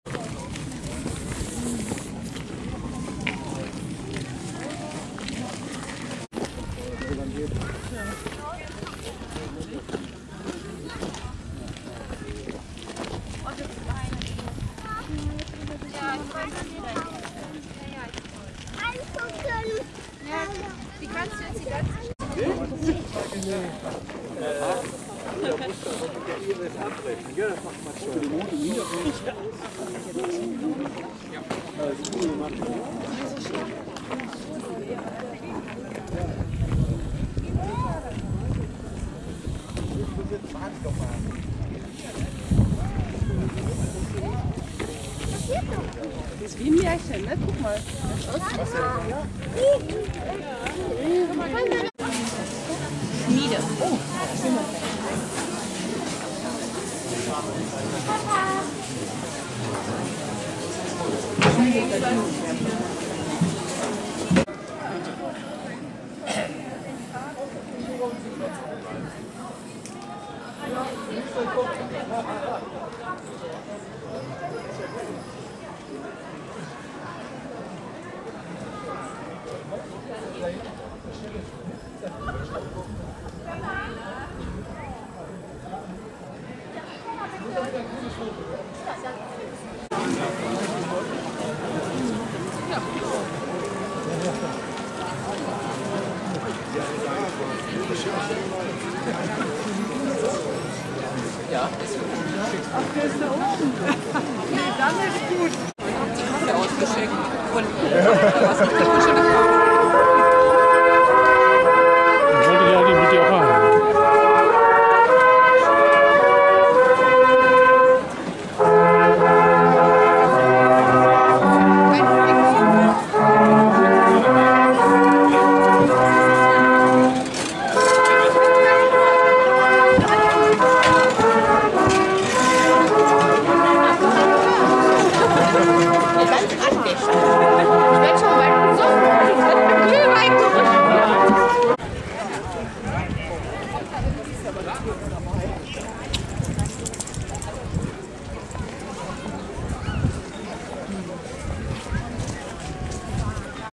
LWL am 04.12.2001: Herrlicher Weihnachtsmarkt im Freilichtmuseum Detmold
Selten so etwas Stimmungsvolles gesehen. Wetter, Stimmung, Glühwein und Dekoration waren sehr gut. Ach ja, die Menschen, Weihnachtsmänner und -männinnen ebenfalls.